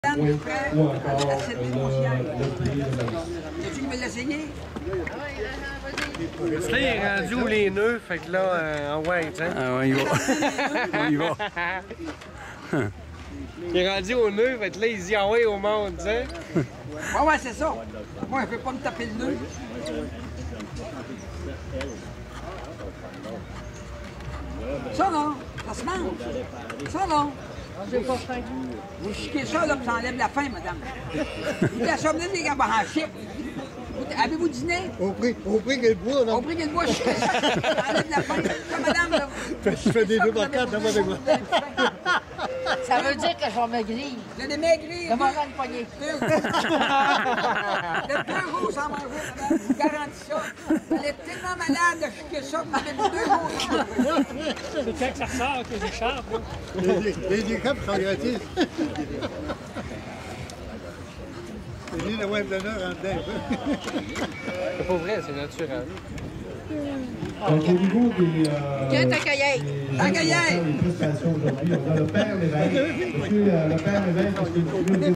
Jean Guilbert au façonnage d'un barreau
Démonstration d'outils de façonnage du bois antique.